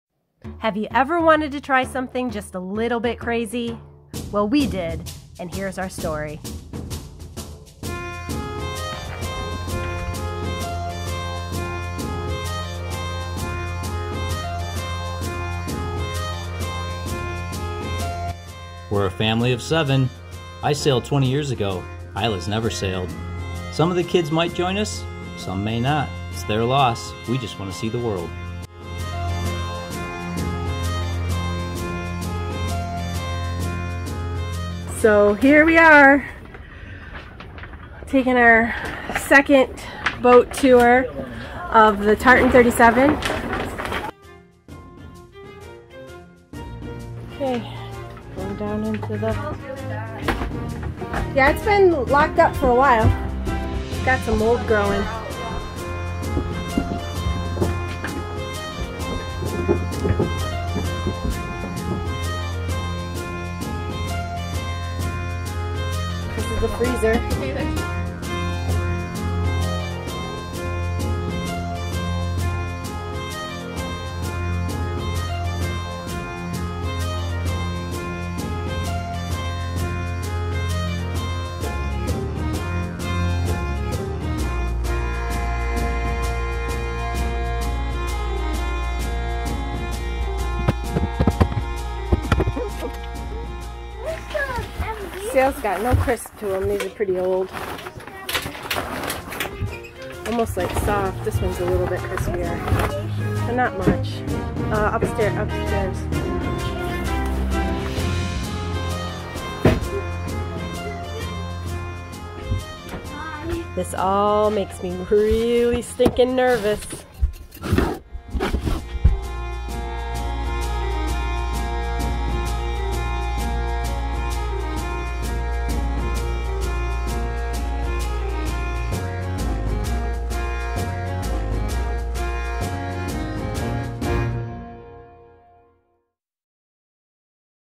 Sailing Finola?
We are entering a new chapter in our lives. As our kids are getting older and starting to leave the nest, we are losing our minds. We found an old 1979 Tartan 37 sailboat and we put an offer on her.  Her name is Finola, and we will decide if that sticks if our offer is accepted. We know very little about what we are doing but hopefully, we will figure it out along the way. You are welcome to join us for laughs and probably tears as we learn to become sailors.